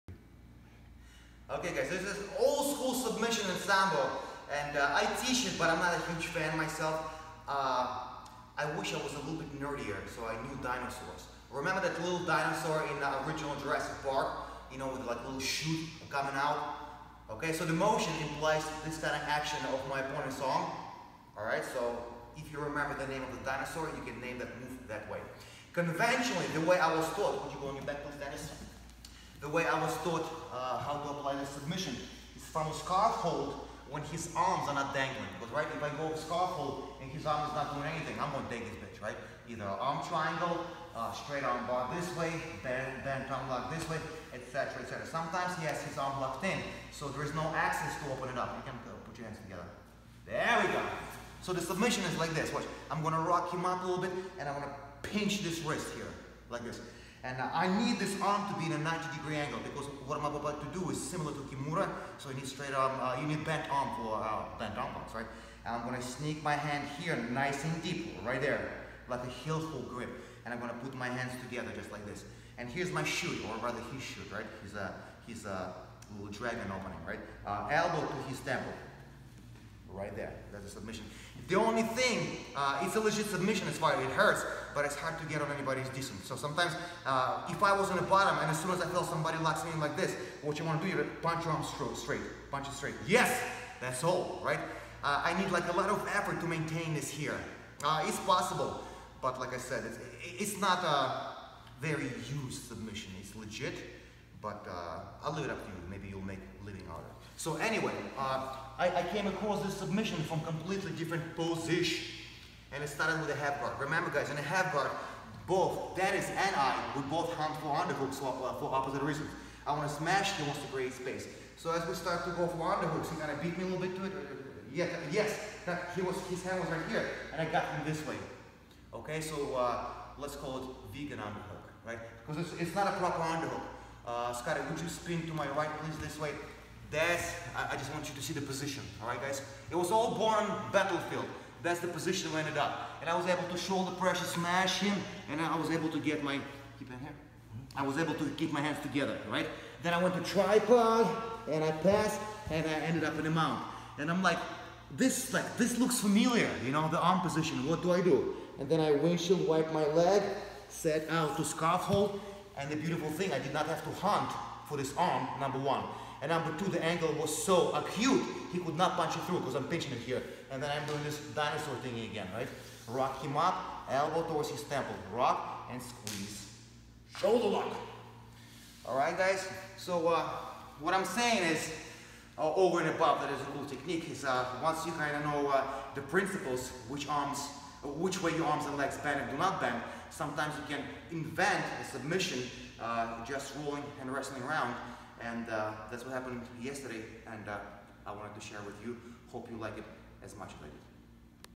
Sambo Shoulder Lock
There is pretty standard shoulder lock in Sambo, typically applied from side pin (scarf hold). Here is a set up I found while trying to pass half guard